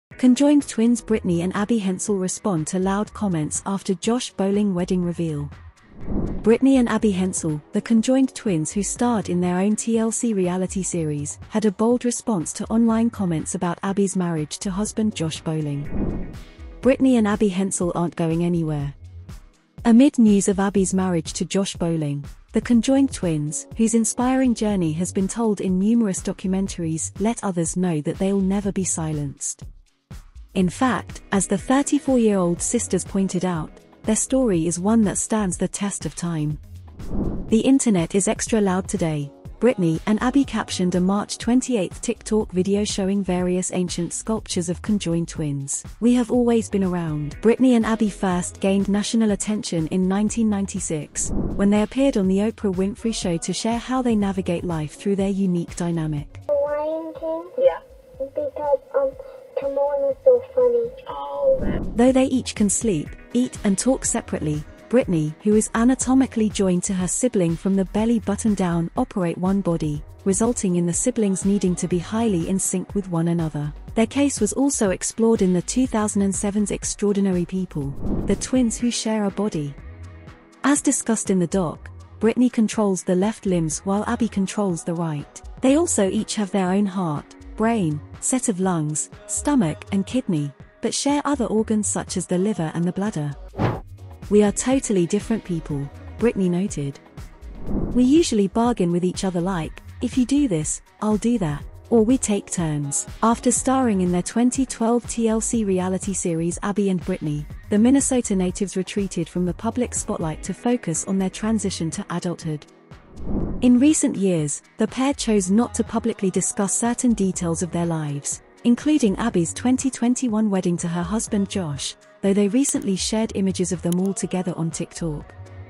The Conjoined Twins, Brittany and Abby Hensel's Bold Reaction | Entertainment News | Flame In Fame
Brittany and Abby Hensel the conjoined twins who starred in their own TLC reality series—had a bold response to online comments about Abby's marriage to husband Josh Bowling. Discover the full details of Brittany and Abby Hensel's outspoken reaction in this exclusive video!

You can the video by these search terms as well